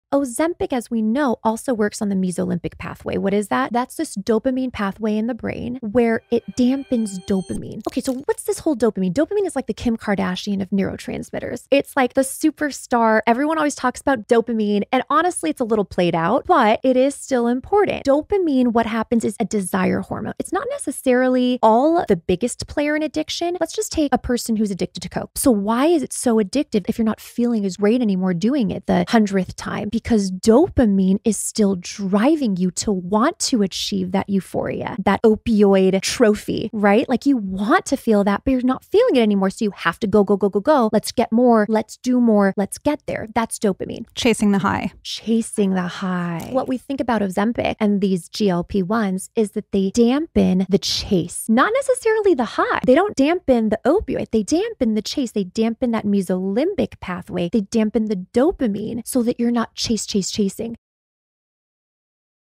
Ozempic & Dopamine: The Science Behind Craving Control
Ever wonder why Ozempic helps curb cravings? It all comes down to dopamine—the superstar neurotransmitter that fuels addiction. But instead of blocking the "high," Ozempic actually dampens the chase. Let’s break down how this works in your brain!

Looking for exclusive wellness tips, beauty hacks, and lifestyle insights you won’t find anywhere else? Don’t miss out!